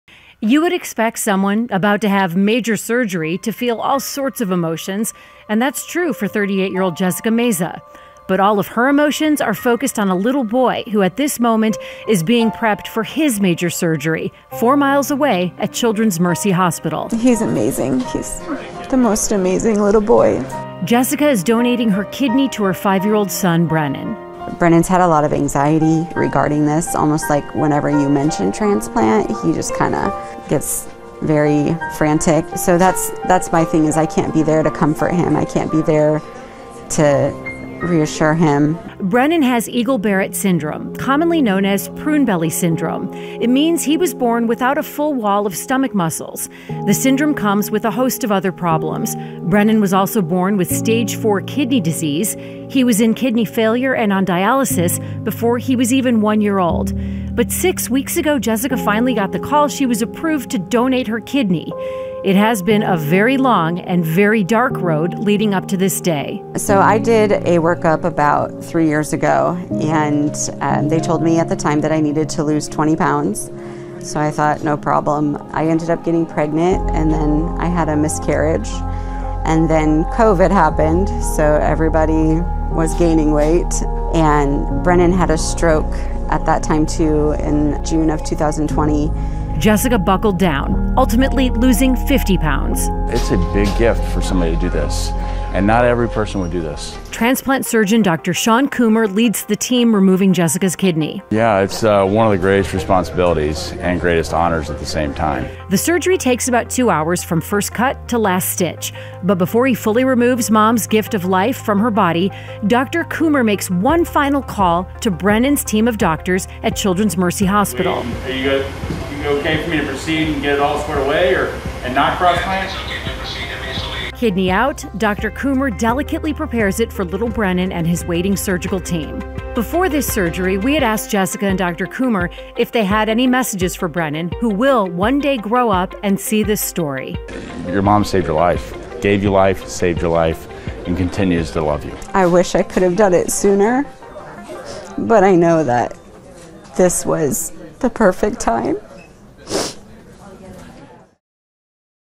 Mother Son Kidney Donation
a heartfelt story, 38-year-old Jessica Meza is donating her kidney to her 5-year-old son, Brennan, who has Eagle-Barrett Syndrome and stage-4 kidney disease. Jessica's journey involves overcoming challenges, including a miscarriage, Brennan's stroke, and the impact of COVID-19. Surgeon Dr. Sean Kumer expresses the significance of Jessica's selfless act. The surgery takes about 2 hours, and Jessica and Dr. Kumer share messages of love and gratitude for Brennan.

or findadoctor.kansashealthsystem.com.